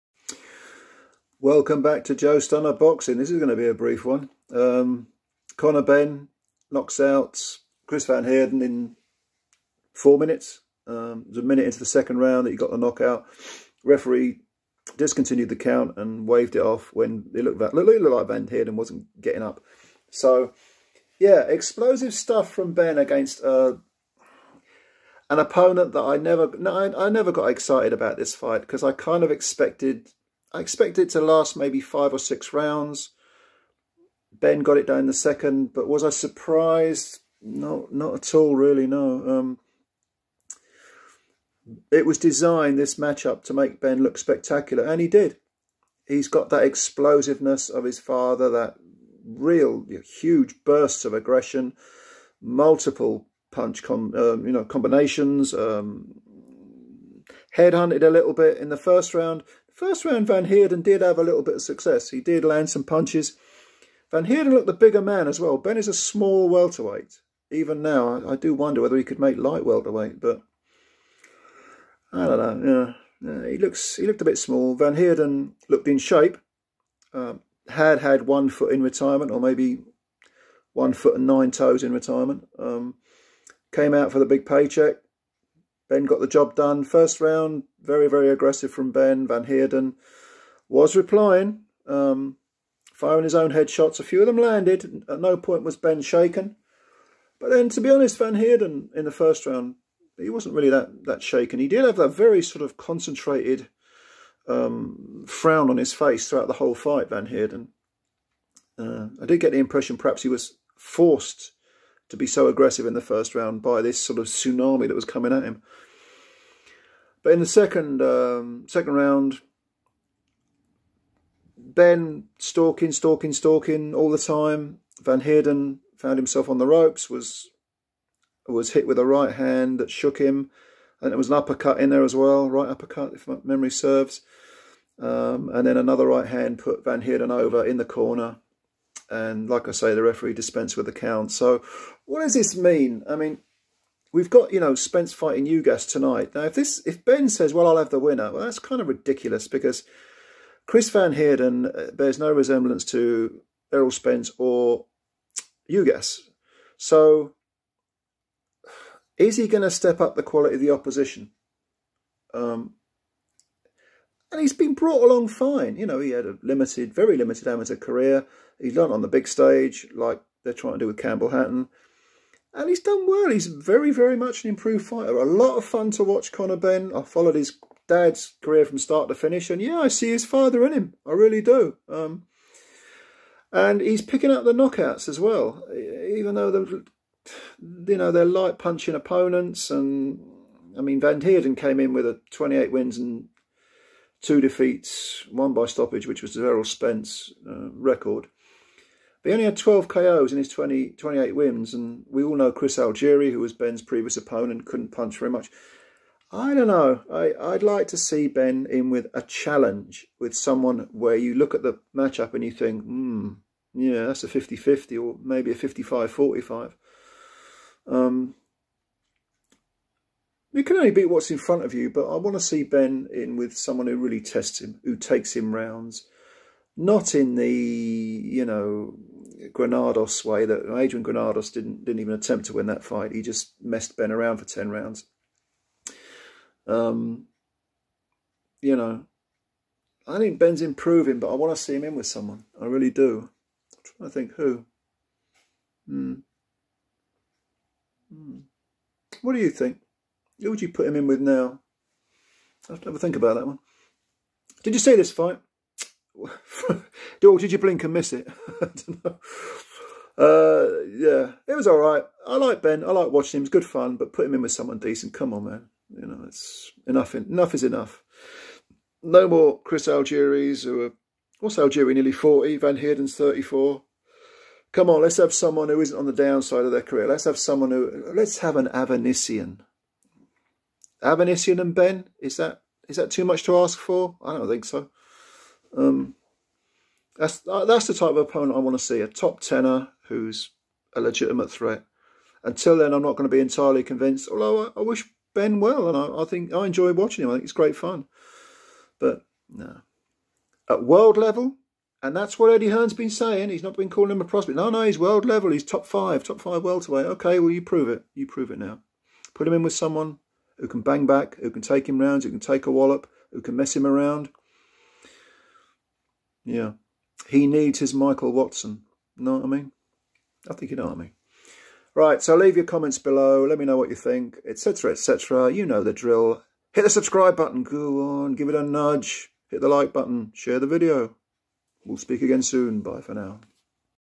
CONOR BENN blitzes CHRIS VAN HEERDEN for a highlight reel KO over another semi-retired opponent.
No surprises here then.  It took Conor Benn four minutes to obliterate the one-foot-in-retirement South African, Chris Van Heerden.  Is Benn still a prospect or a world class contender?  If it's the latter, Hearn and DAZN need to step up the opposition to non-punchers who are seeking their pipes and slippers.